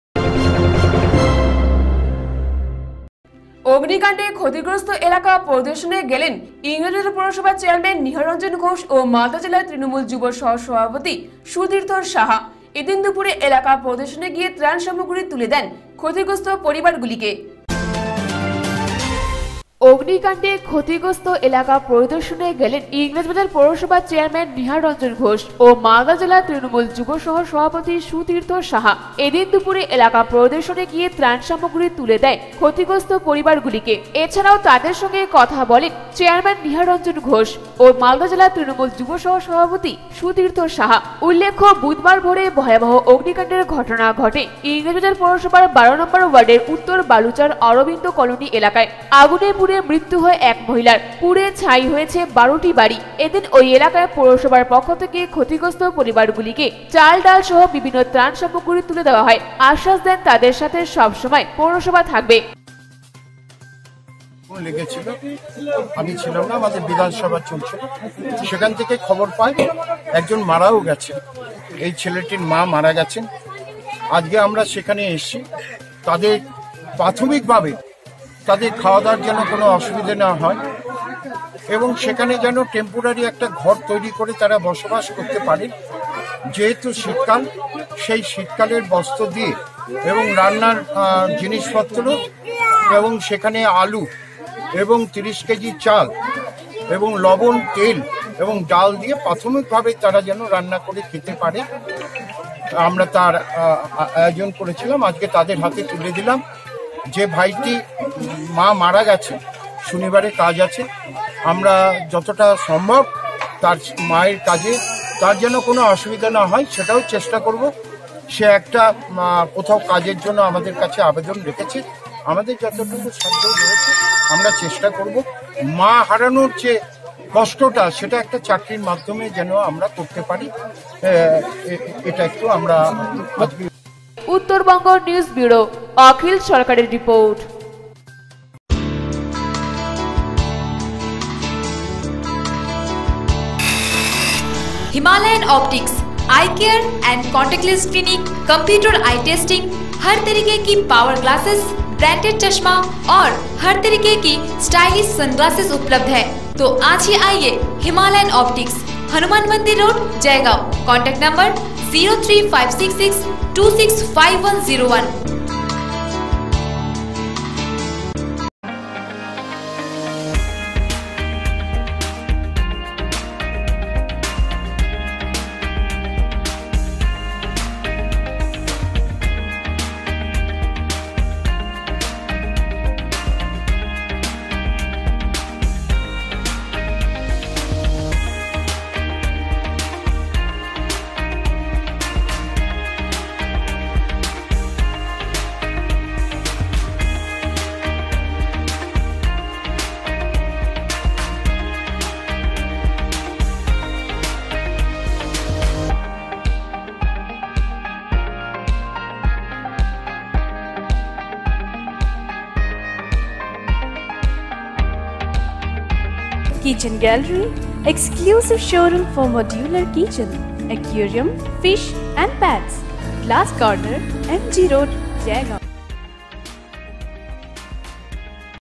মালদা শহরের আগুনে পুড়ে বিধ্বংসী পরিবারগুলির পাশে দাঁড়ালেন ইংরেজবাজার পৌরসভার চেয়ারম্যান
অগ্নিকান্ডে ক্ষতিগ্রস্থ এলাকা পরিদর্শনে গেলেন ইংরেজবাজার পুরসভার চেয়ারম্যান নিহার রঞ্জন ঘোষ ও মালদা জেলা তৃণমূল যুব সহ-সভাপতি সুতীর্থ সাহা,এদিন দুপুরে এলাকা পরিদর্শনে গিয়ে ত্রাণ সামগ্রি তুলে দেয় ক্ষতিগ্রস্থ পরিবারগুলিকে